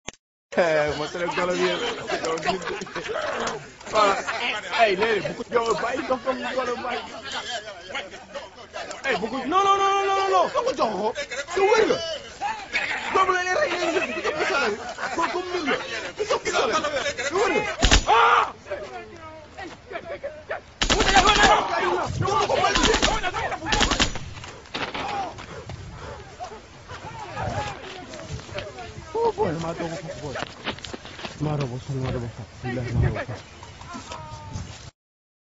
Rowdy monkey
Monkey getting slapped